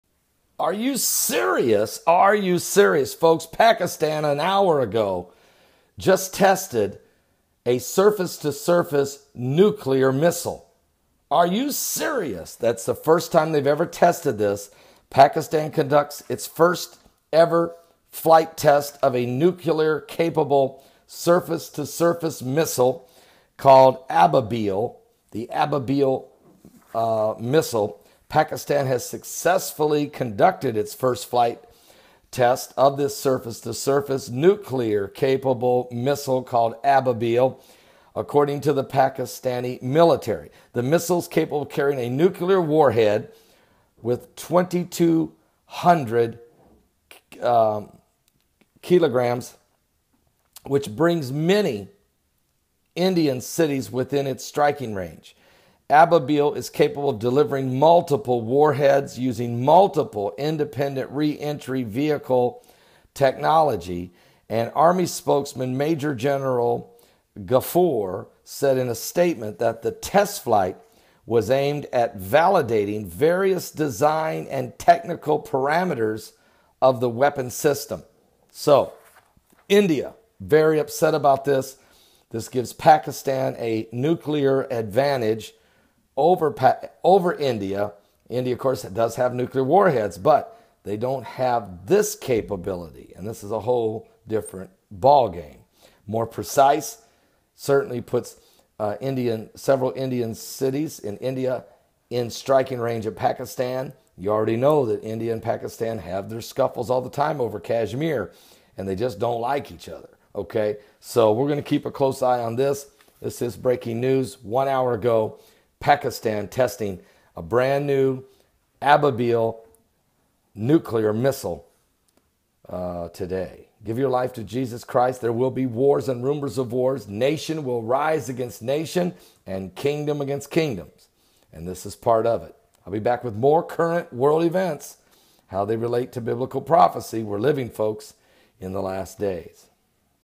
BREAKING: "Pakistan Test New Nuclear Missile "Ababeel"
Pakistan has just tested a "Surface to Surface Nuclear Missile" that threatens India also Help Us Spread The Word